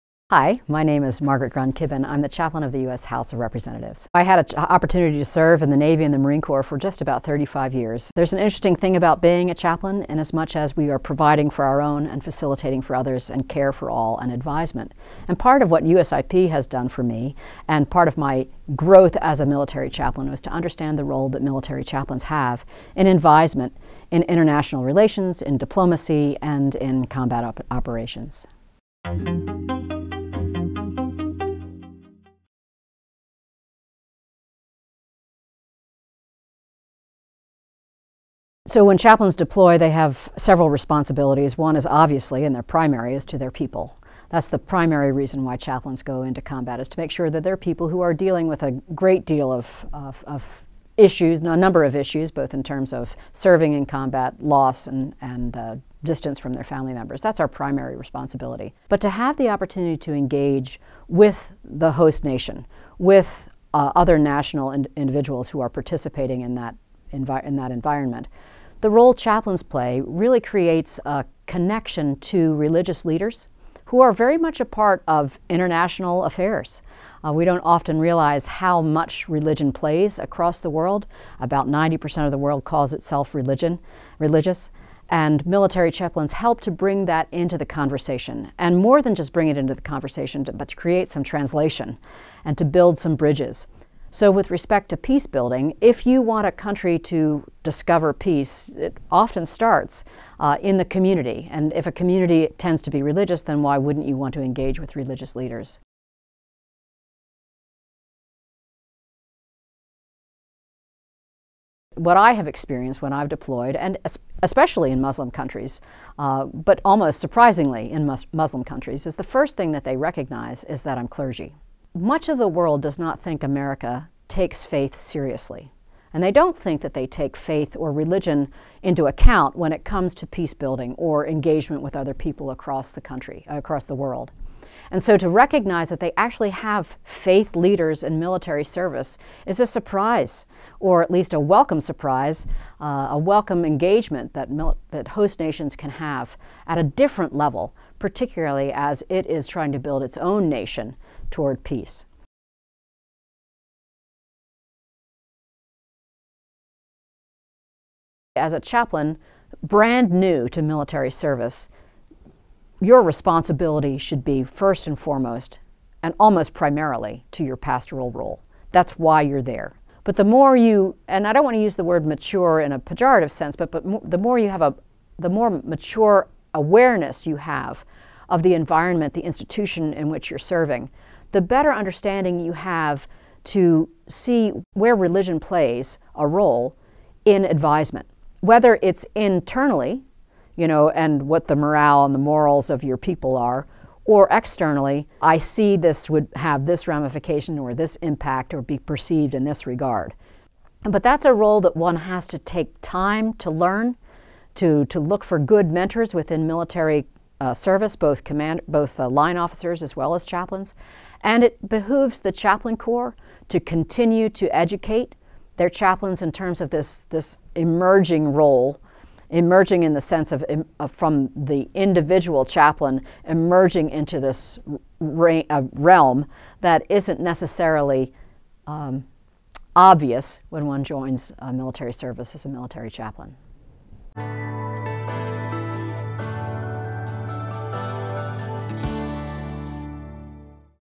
The Latest @ USIP: How Military Chaplains Can Open Doors for Peacebuilding
While often overlooked, religious leaders are very much a part of international affairs. And in conflict environments, religious leaders can offer guidance, open doors and build bridges both within their own communities and between adversaries. Rear Admiral Chaplain Margaret Grun Kibben, the U.S. House of Representatives chaplain and former USIP senior fellow, discusses the role of faith leaders in helping end conflict abroad, her experience engaging Afghan religious leaders and the lessons she hopes to pass on to future chaplains.